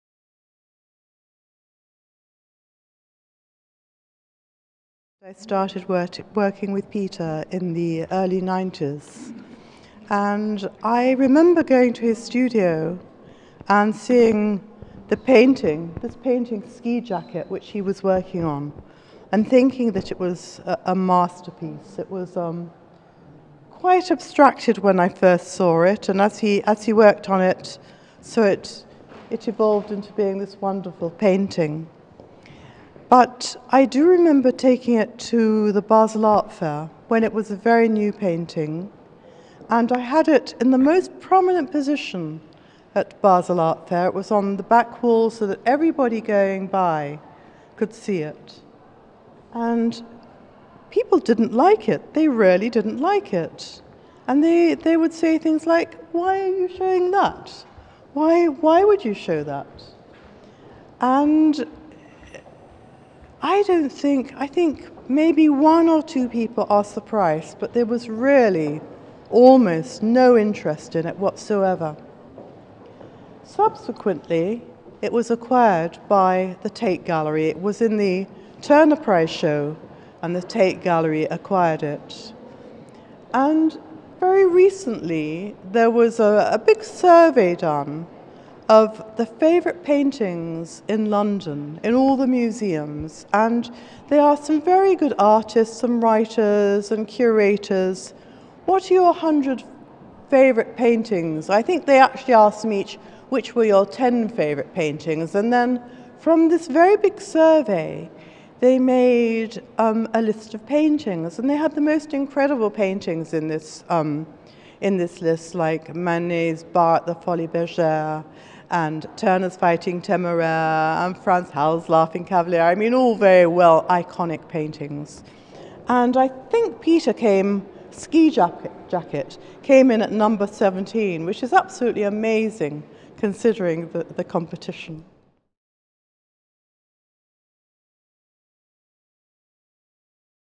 Victoria Miro on Peter Doig's painting Ski Jacket at Fondation Beyeler
PETER DOIG. NOVEMBER 23, 2014 – MARCH 22, 2015

Peter Doig (*1959) is one of the most fascinating artists of today. The Fondation Beyeler presents an exhibition of his most important oil paintings and experimental works on paper, as well as a monumental new mural. Doig, who was born in Edinburgh, grew up in Trinidad and Canada and now divides his time between Trinidad, London and New York, creates potently colourful works that primarily show people and architecture in landscapes of seductive beauty. Below the surface of this paradise lies a sense of melancholy, loneliness, tension and calamity. These pictures are memories of and yearnings for a wondrous dream world that seems strange and at the same time familiar to us. Peter Doig’s works have their starting point in private photographs and images from the media and movies, and combine pop culture and art history in an enigmatic fashion.